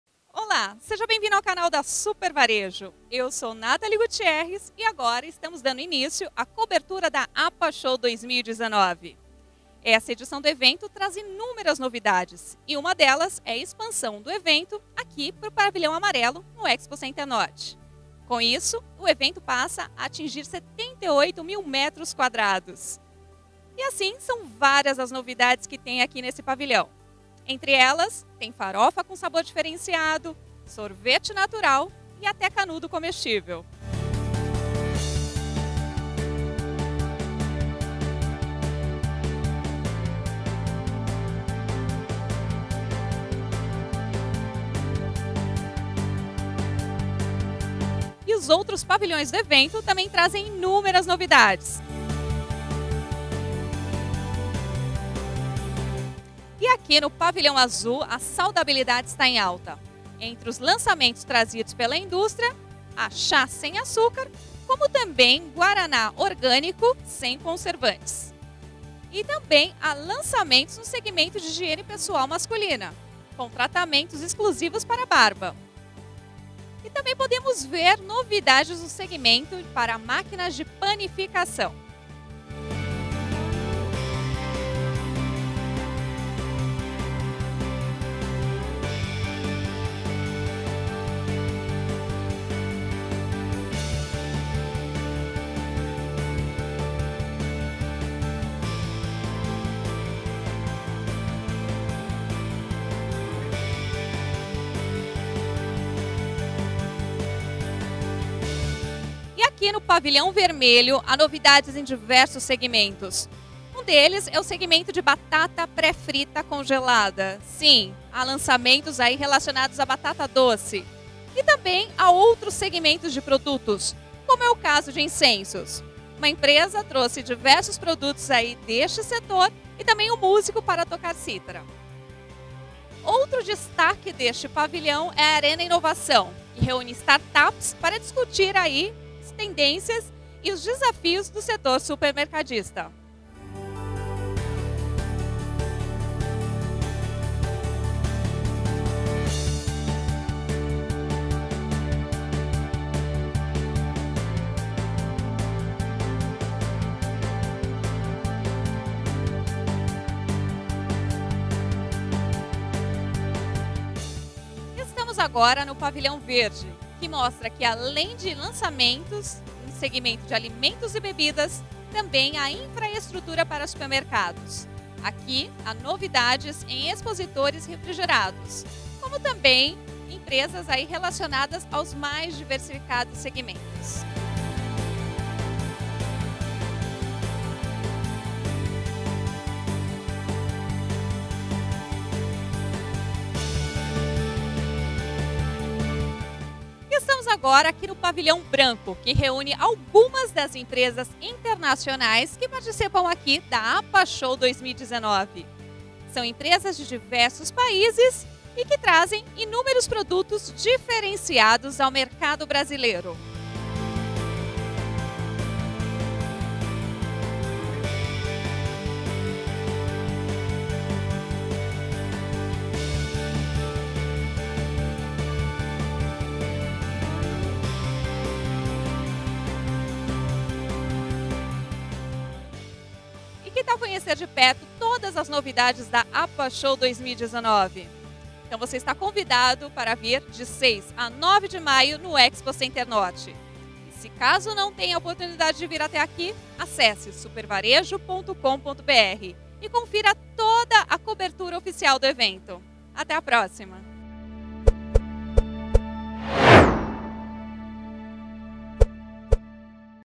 Pavilhões APAS Show 2019
Destaques dos cinco pavilhões da APAS Show 2019, o maior evento supermercadista do mundo.